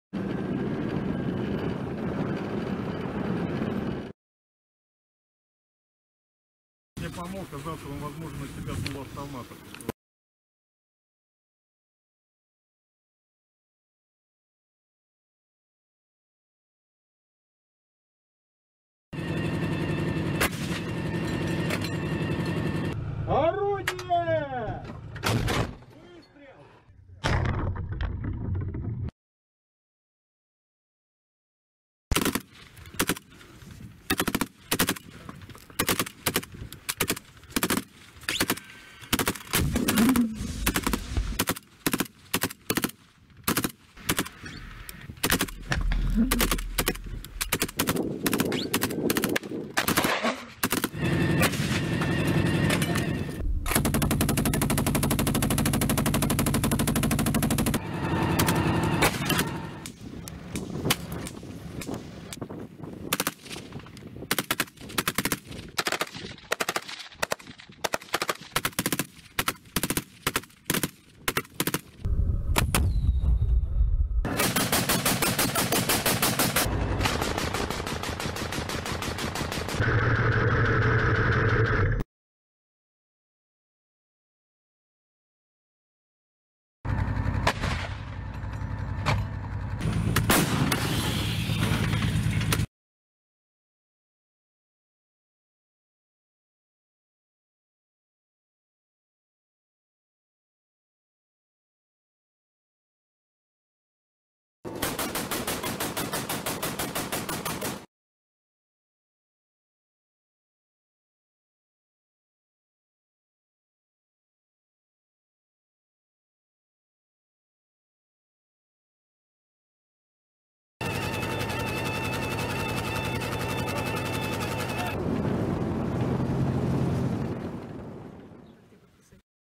Ukrainian offensive towards Kherson smashed by russian forces!
Footage of russian forces stopping an ukrainian army offensive with armored vehicles and soldiers north of Liubomyrivka, Mykolaiv region.